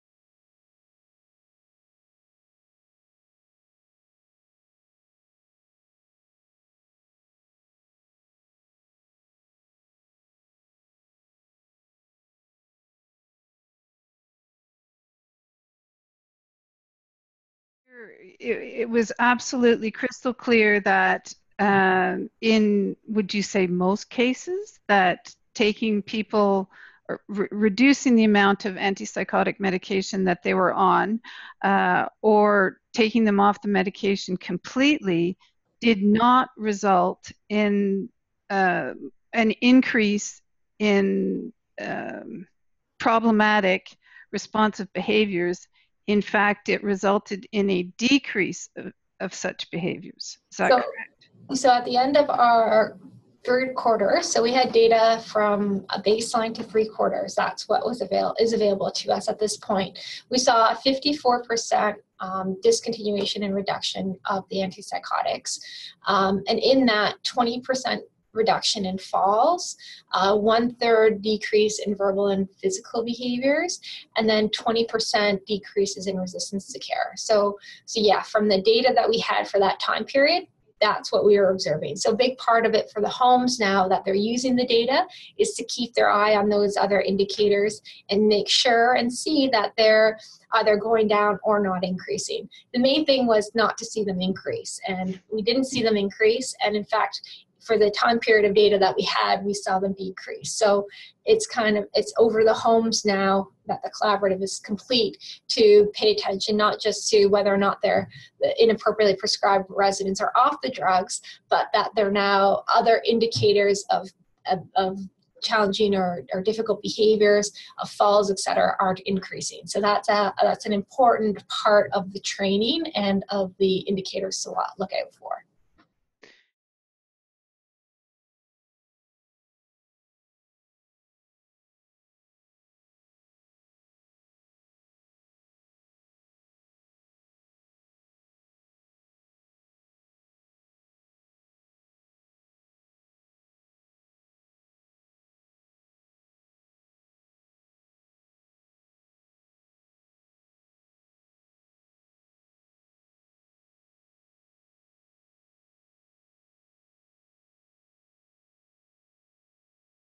What happens when antipsychotic medication is decreased or stopped
A clear answer to a simple question regarding the results achieved by a collaborative initiated by the Canadian Foundation for Healthcare Improvement. Kaye Phillips, Senior Director of the CFHI answers the question posed by dementia care advocate Susan Macaulay of